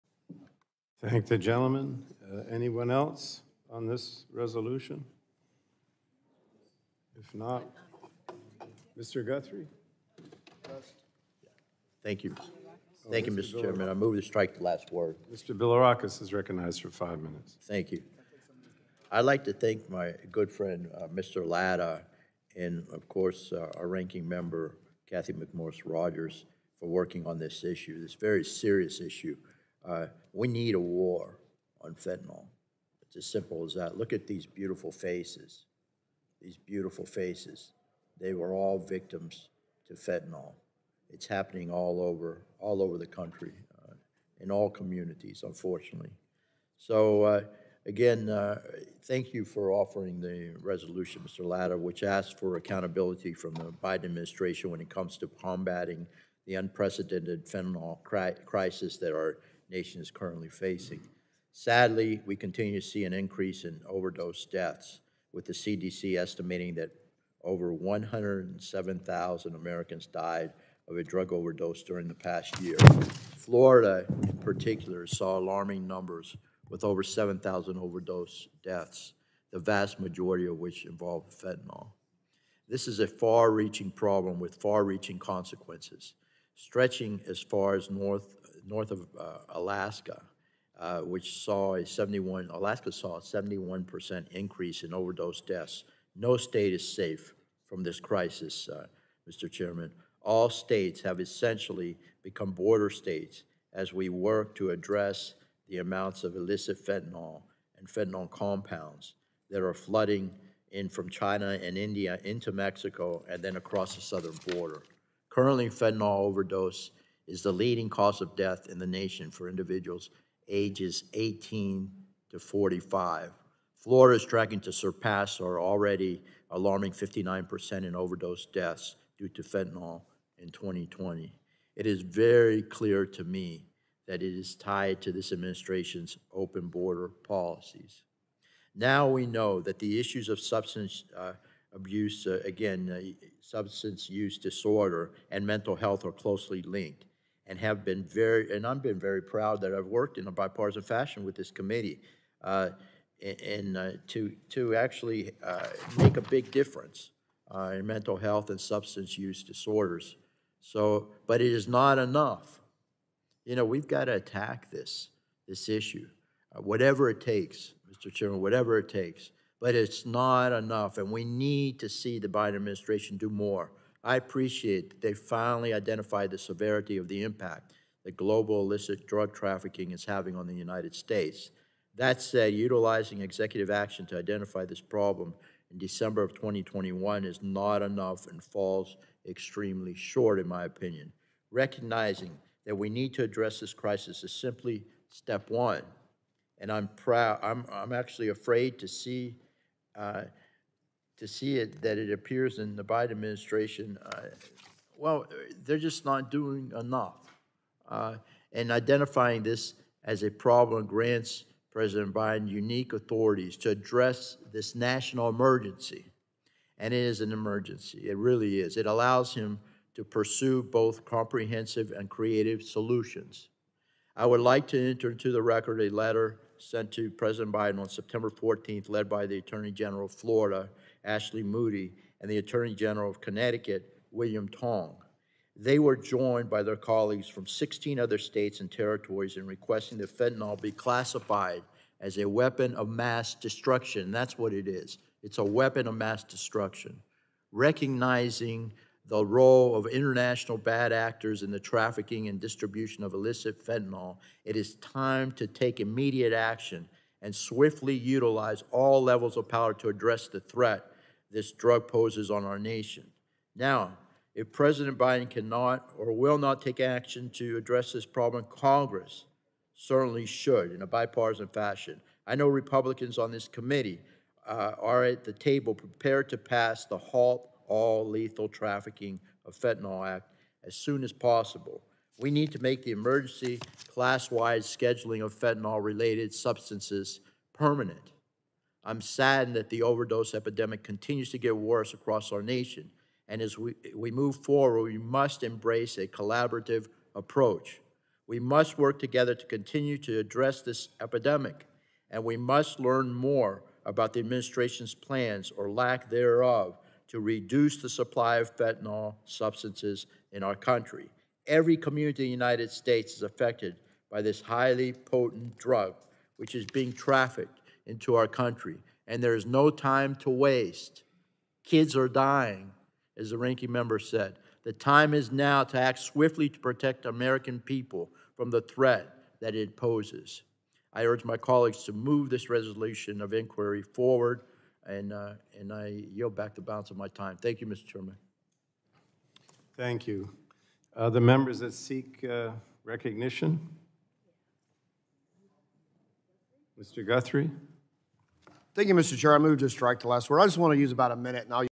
'It's A Weapon Of Mass Destruction': GOP Lawmaker Says Biden Not Addressing Major Threat To US
At Thursday's House Energy Committee hearing, Rep. Gus Bilirakis (R-FL) tied the fentanyl death crisis to President Biden's border policies.